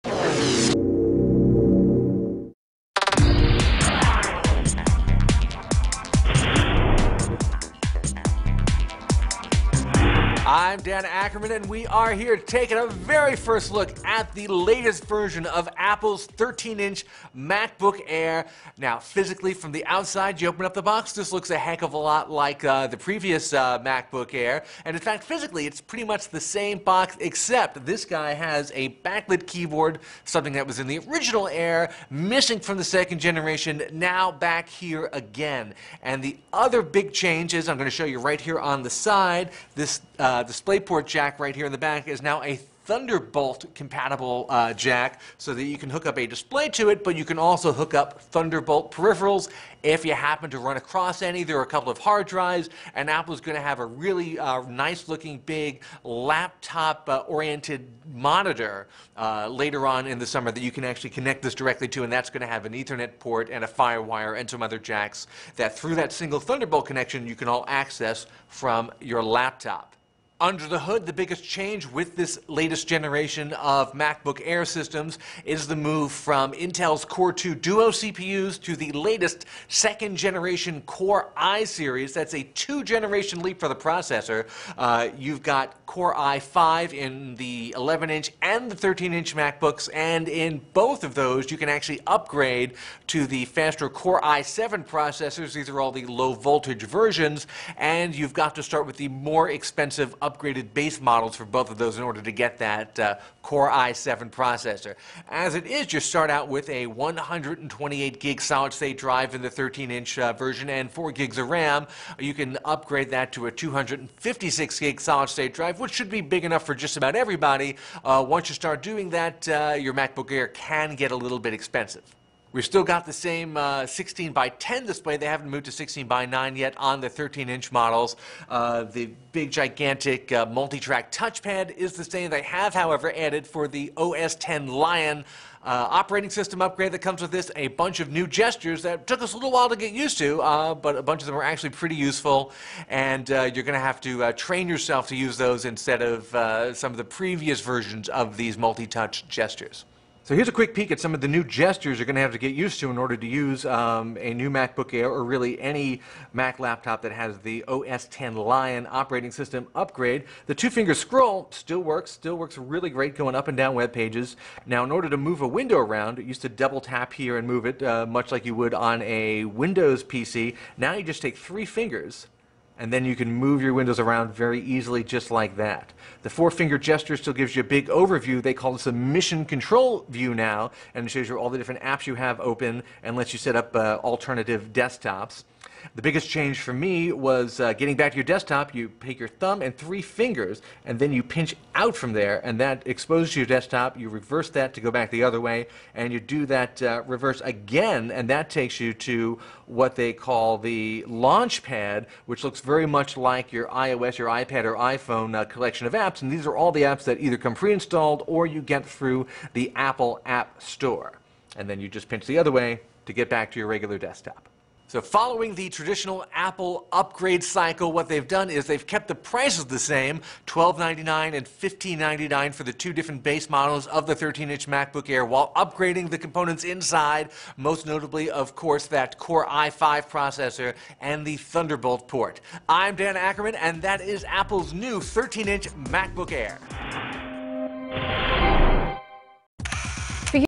Apple New Macbook Air 13 inch
씨넷 TV는 애플이 지난달 20일 새로 출시한 '맥북에어 13인치'의 변화된 점에 대해 상세히 소개했다.

새 운영체제인 'OS X 라이온'이 탑재된 신형 '맥북에어'는 인텔 2세대 코어 프로세서가 탑재되고 백라이트 키보드, 썬더볼트 포트 탑재 등 전작에 비해 한층 성능이 개선됐다. 또한 맥북에어 특유의 얇고 유려한 디자인은 물론 가격까지도 전작과 비슷하게 출시돼 많은 인기를 모을 것으로 예상된다.